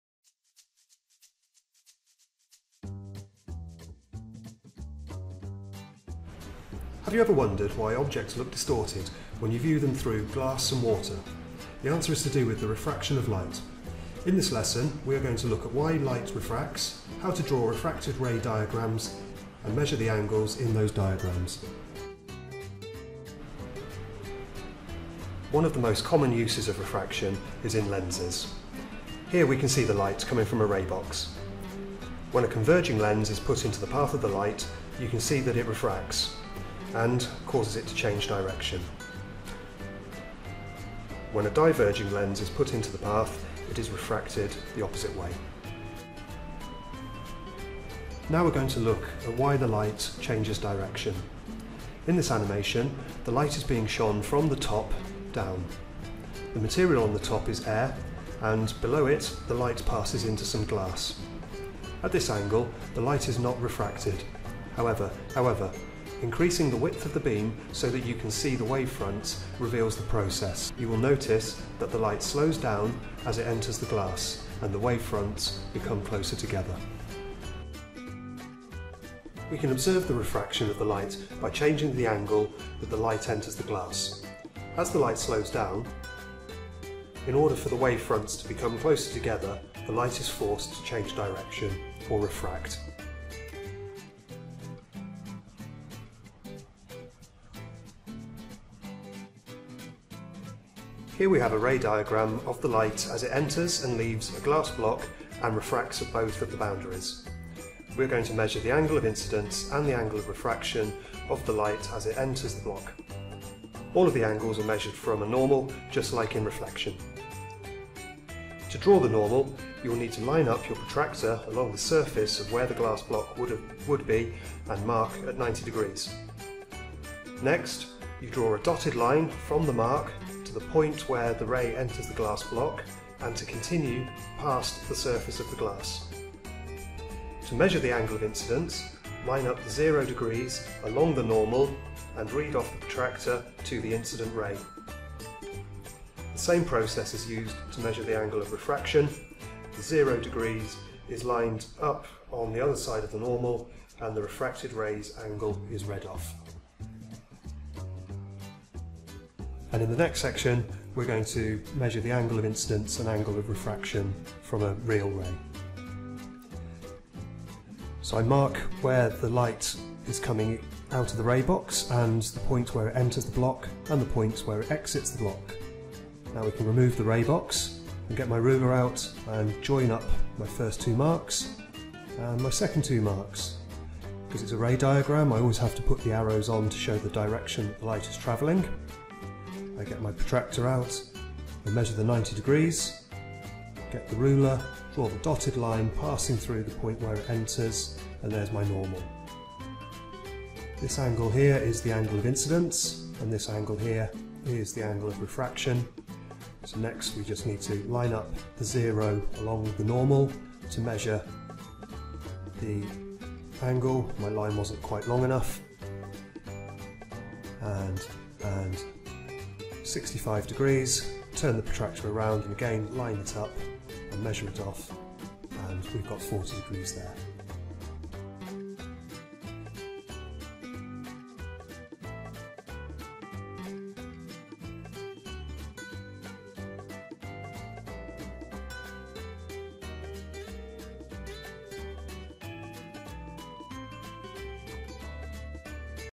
Measuring refraction
Instructional video to teach students how to measure angles of incidence and angles of refraction